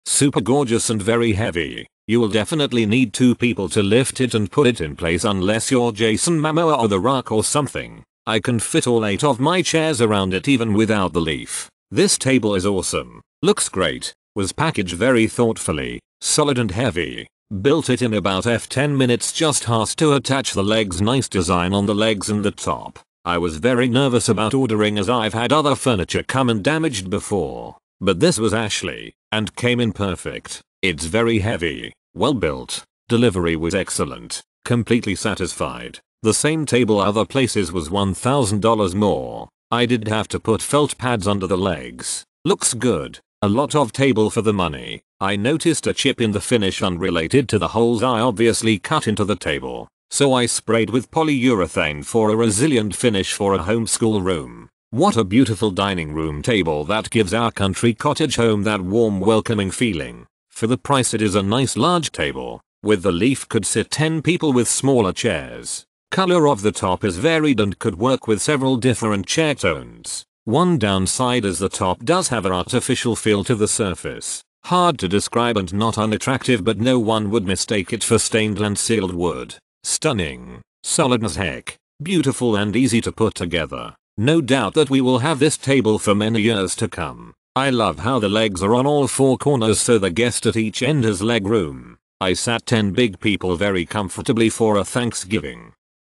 Signature Design by Ashley Realyn French Country Dining Extension Table, Seats up review
Signature Design by Ashley Realyn French Country Dining Extension Table, Seats up to 8, Chipped White

Main Features:
- DINING ROOM TABLE: Bon appetite. This dining room extension table has all the right ingredients for a French country-inspired feast
- HANDSOMELY CRAFTED: Made of veneer, wood and engineered wood with some cast resin components. Antiqued two-tone finish with separate extension leaf which seats up to eight comfortably
- COTTAGE DESIGN: This dining room extension table has all the right ingredients for a French country-inspired feast for the eyes
- EXTENSION TABLE: Measures 42" W x 96" D x 30" H with leaf. Table depth without leaf 78"
- ASSEMBLY REQUIRED: Easy-to-follow instructions, tools and hardware included